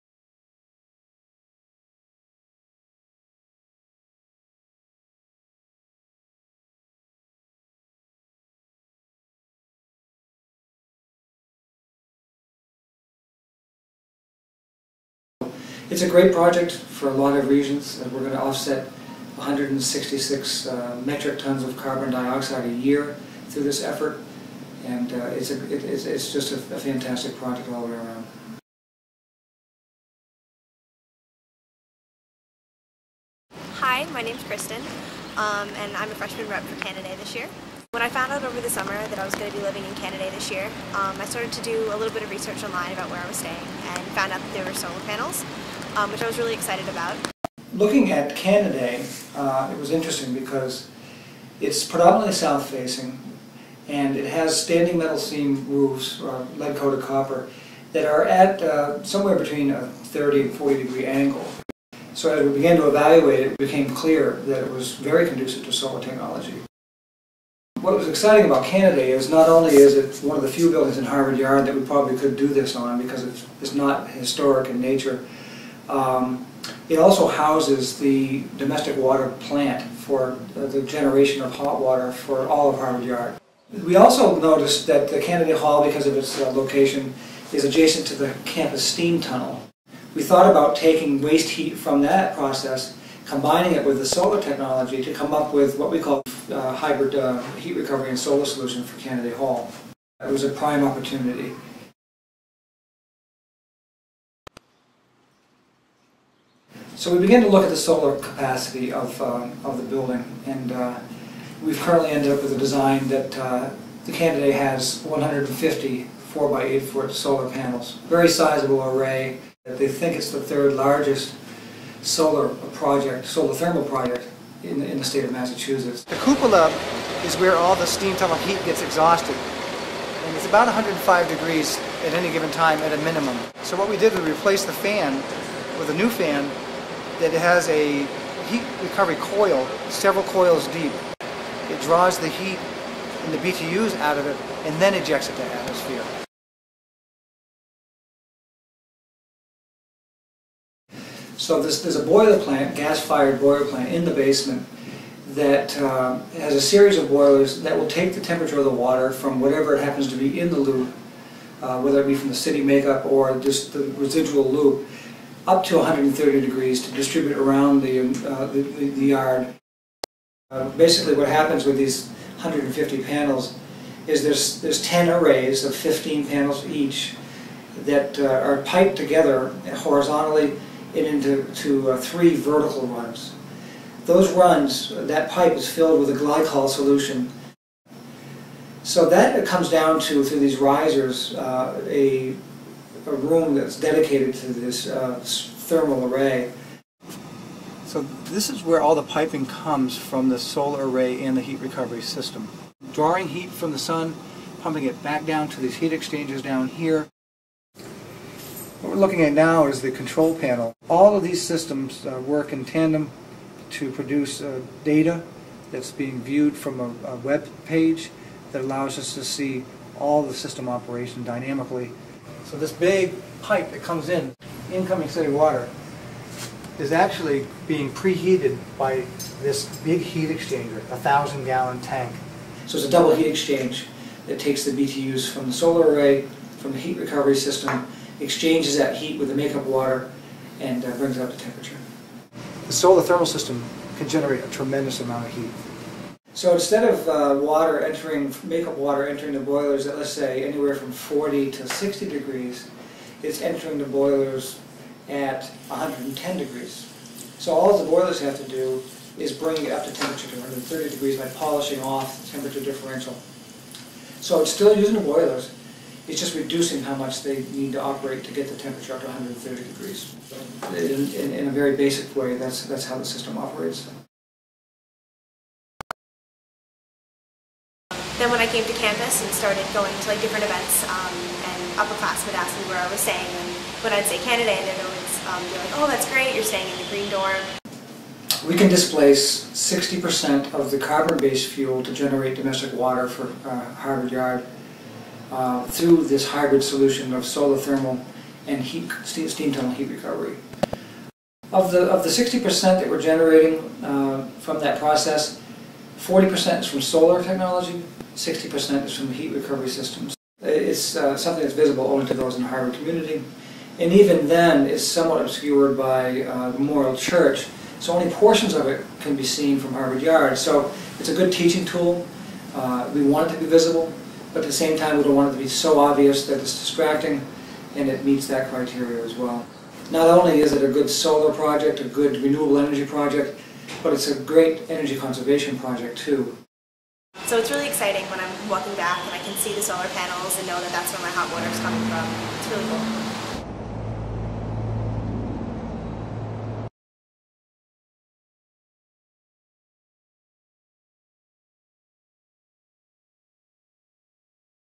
Canaday Solar and Heat Recovery System
In 2010, Harvard University installed a solar and steam tunnel heat recovery system at Canaday Hall that is expected to supply at least 60% of domestic hot water needs for all buildings in Harvard Yard.  Check out this insider's tour of the project featuring Jay Phillips, senior director of operations for FAS, and Freshman REP Kristen Wraith.  Thanks to student Ana Martin-Caughey for producing the video!

Thanks to